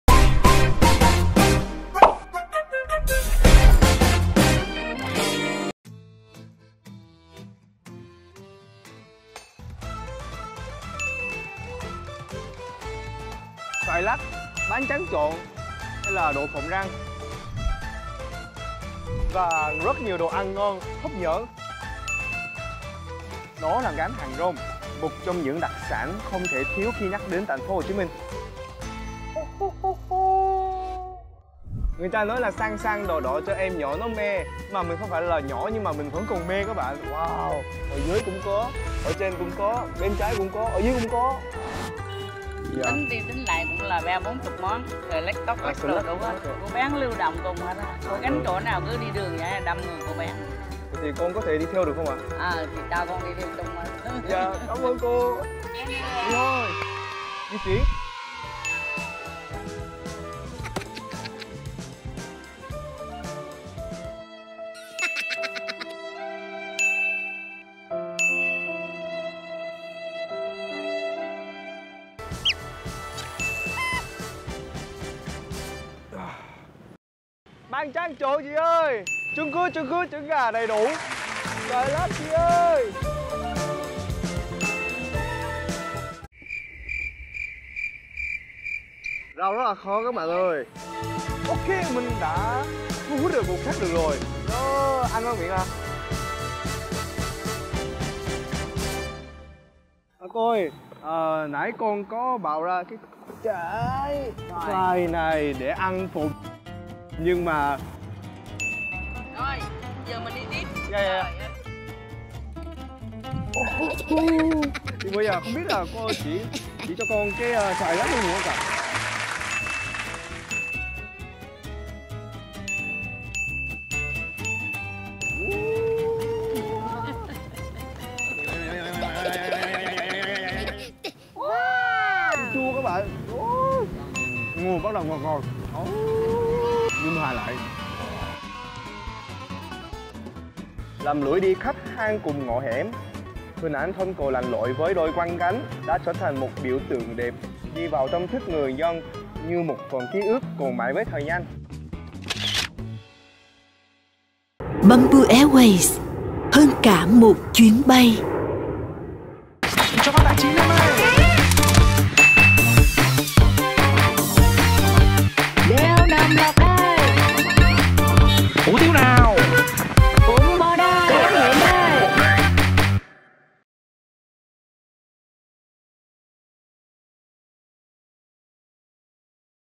1 ngày gánh hàng rong của Hot boy Hàn Quốc Woossi | Ẩm thực đường phố - VTV Travel
Woossi đã khổ sở với gánh hàng rong thế này đây !!!

►Nhà tài trợ: FLC Group